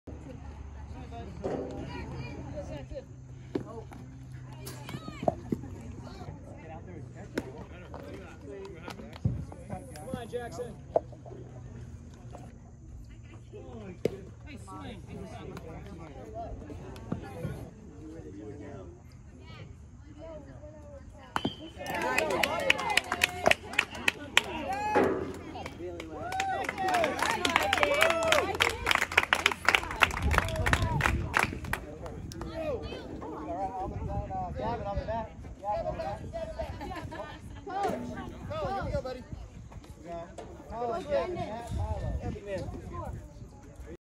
Jack’s RBI
8 yr old Jack playing in the 10-11 yr old league.